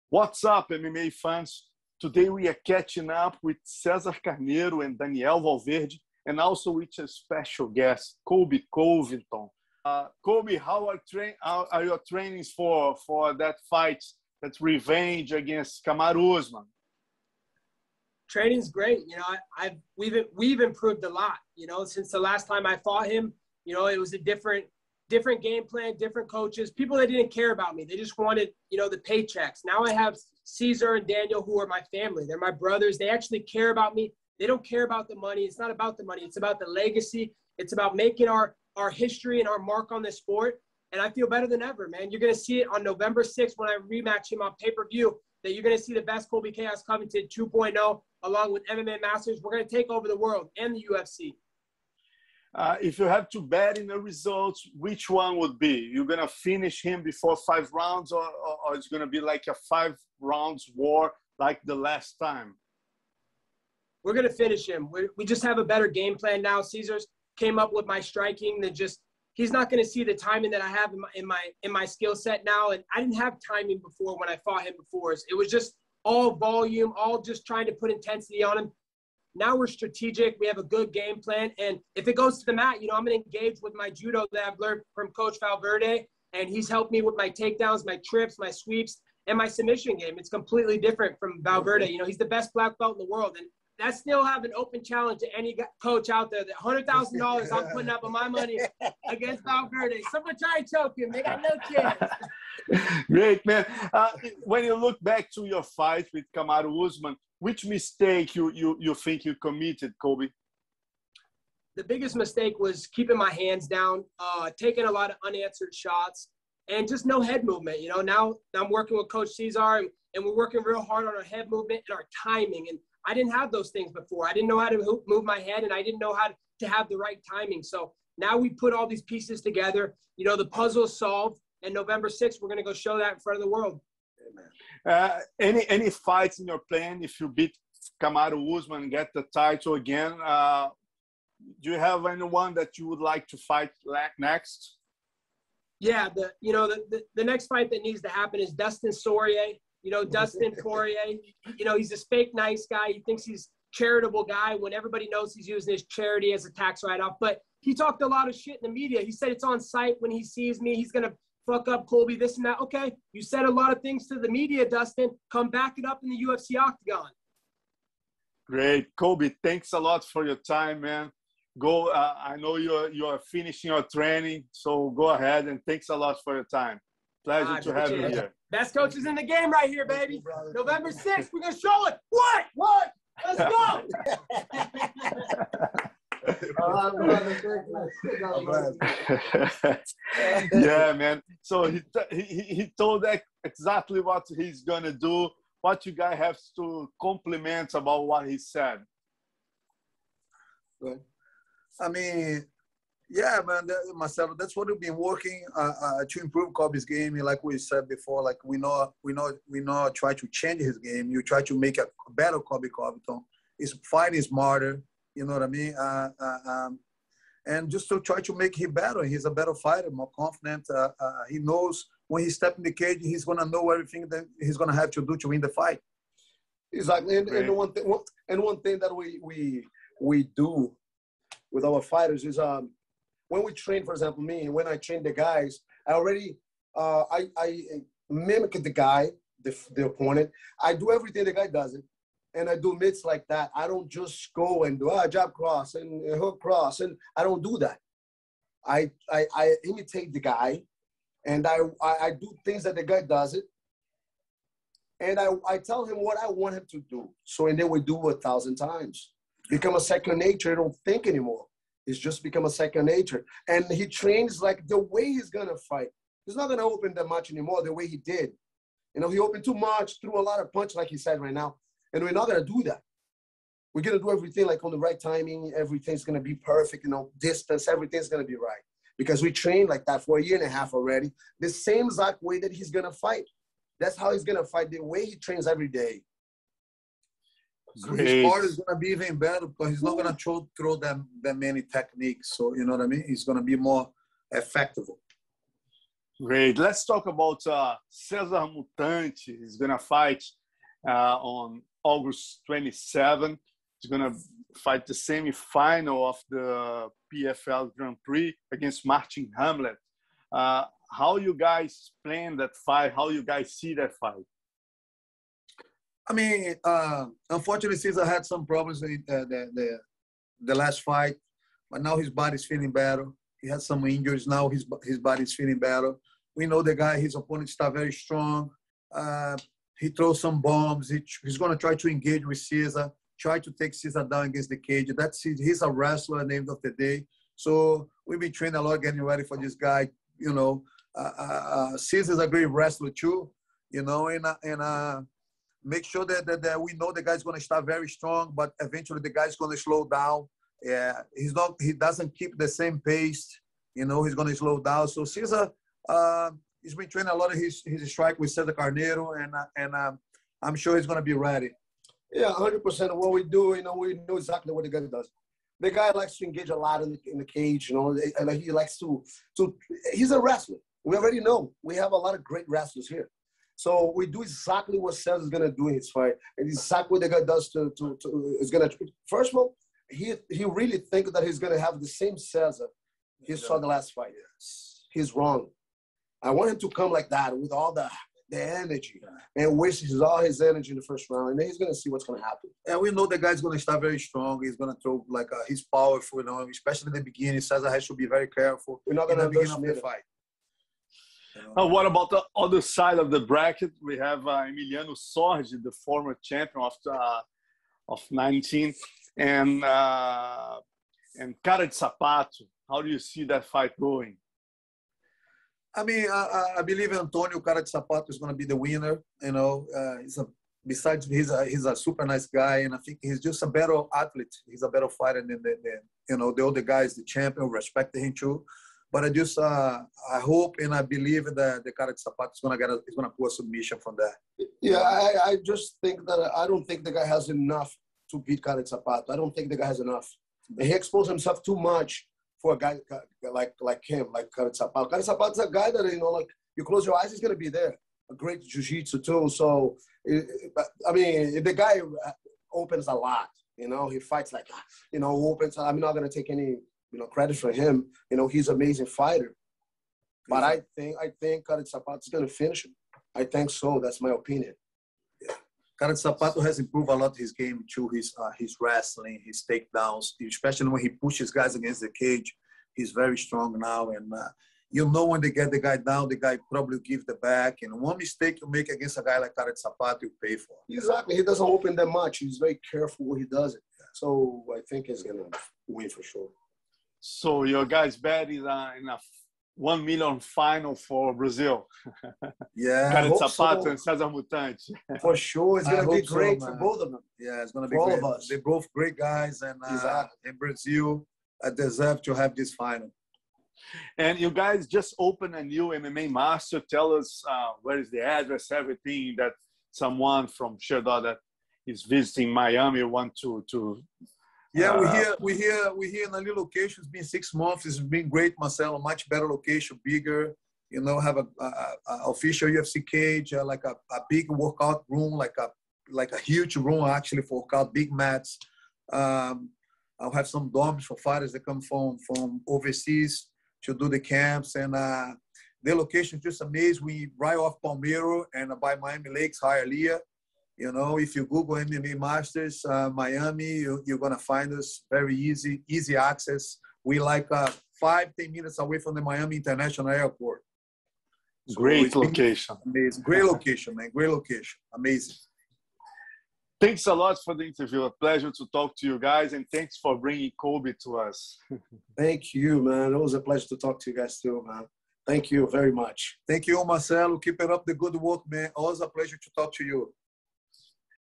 Colby Covington: I Will Finish Kamaru Usman at UFC 268
Scheduled to face Ultimate Fighting Championship welterweight king Kamaru Usman on Nov. 6 at UFC 268, Colby Covington talked to Sherdog’s Marcelo Alonso about the rematch and promised a totally different outcome from the first fight.

Besides promising to finish the champion en route to capturing the title, Covington predicted that his next opponent will be Dustin Poirier.

In the sequence, MMA Masters head coaches Cesar Carneiro and Daniel Valverde, talked about how Covington has evolved from the first fight and analyzed the confrontation between Cesar Ferreira and Martin Hamlet in the Professional Fighters League Playoffs on Aug 27 at PFL 2021 9.